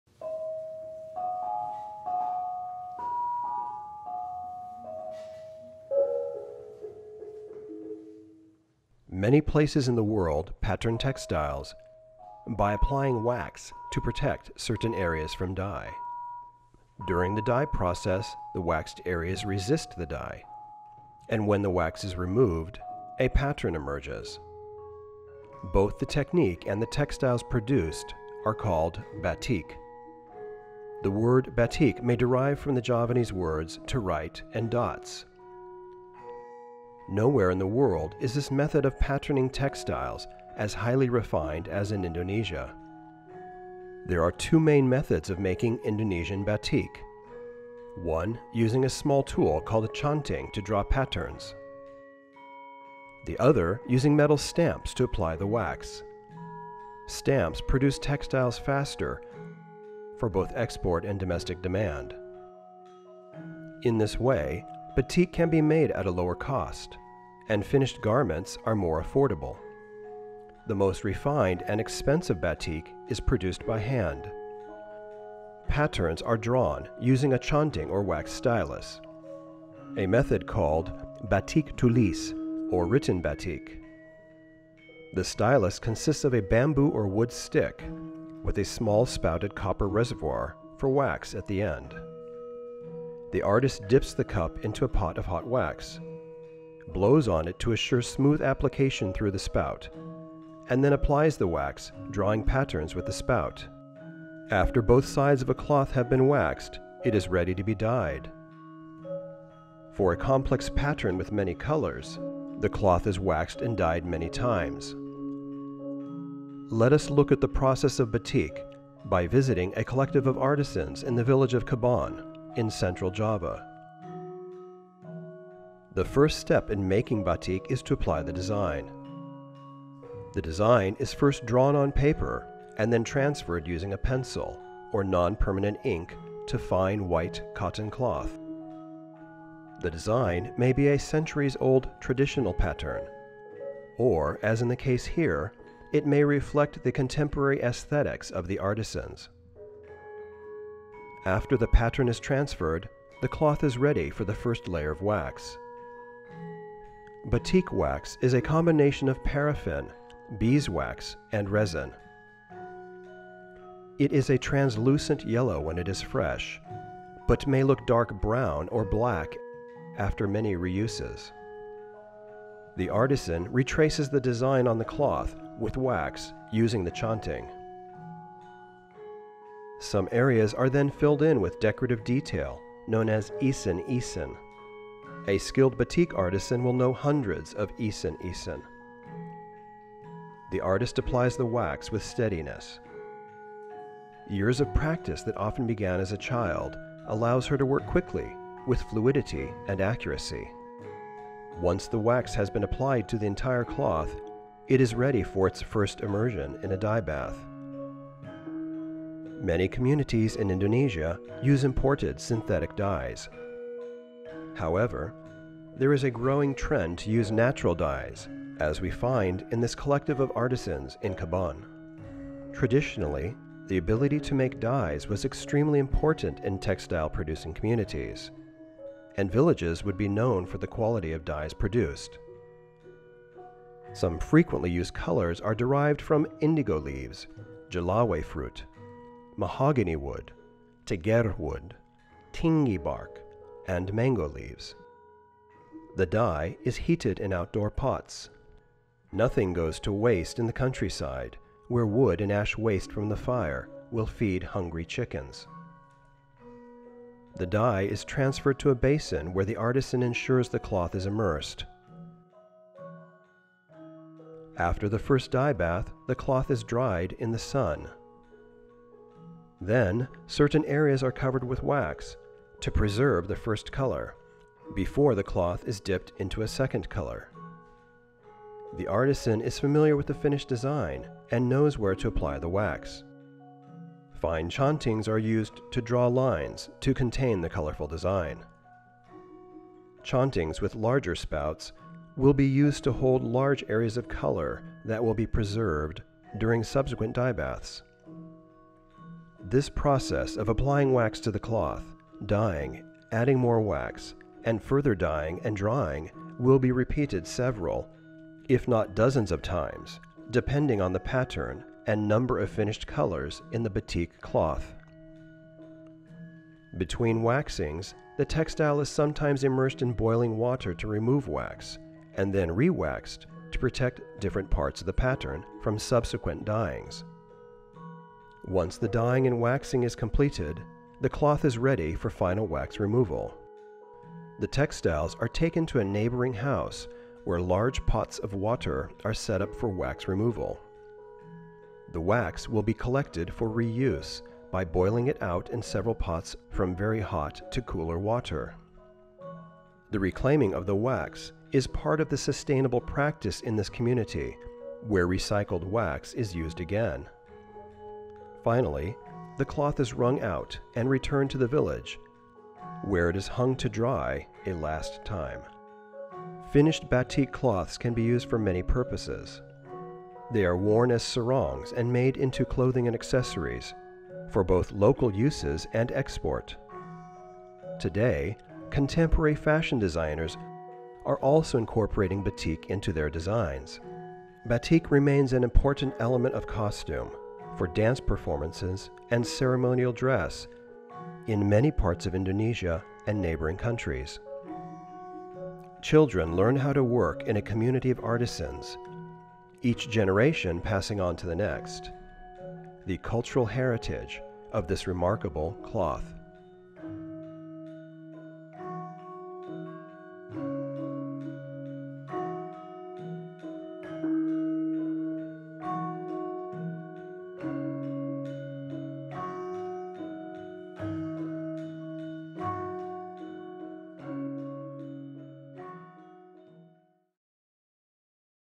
Batik of Java: A Centuries Old Tradition
Watch Javanese artisans make batik and learn more about their refined techniques. Many places in the world pattern textiles by applying wax to protect certain areas from dye. During the dye process, the wax areas resist the dye, and when the wax is removed a pattern emerges. Both the technique and the textile produced are called batik. Nowhere in the world is this method of patterning textiles as highly refined as in Indonesia.